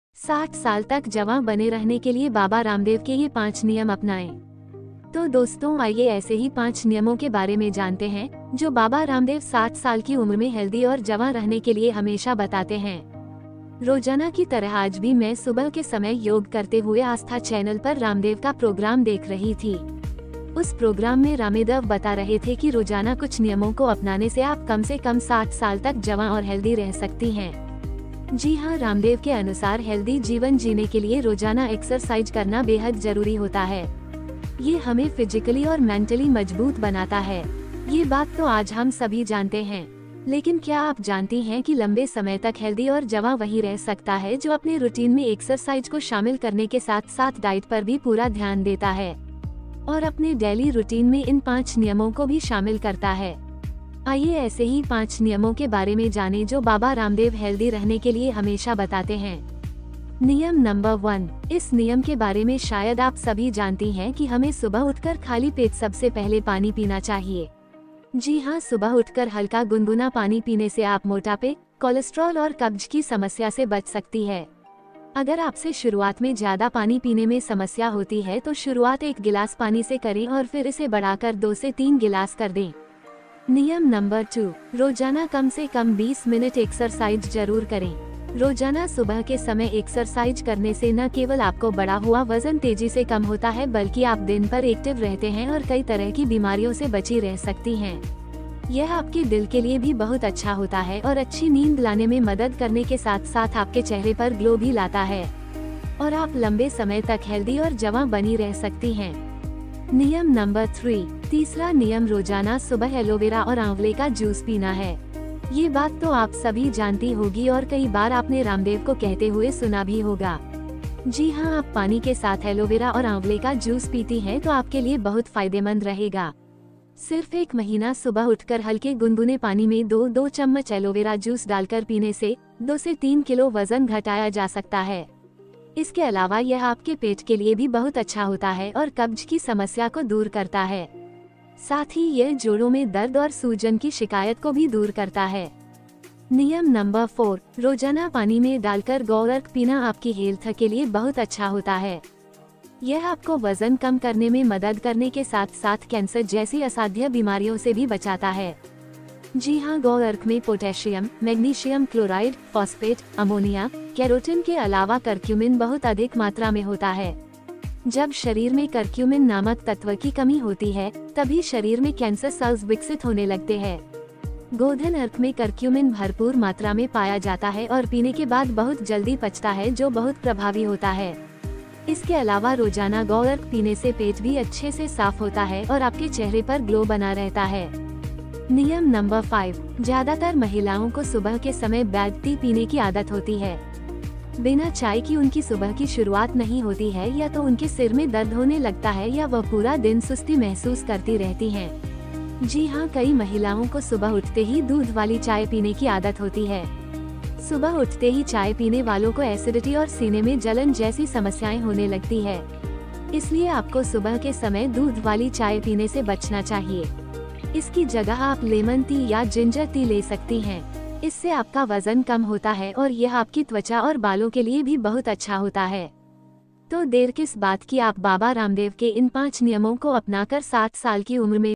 60 साल तक जवां बने रहने के लिए बाबा रामदेव के ये 5 नियम अपनाएं । बाबा रामदेव टिप्स।
60 साल तक जवां बने रहने के लिए बाबा रामदेव के ये 5 नियम अपनाएं । बाबा रामदेव टिप्स।

Follow these 5 rules of Baba Ramdev to remain young till 60 years. Baba Ramdev Tips.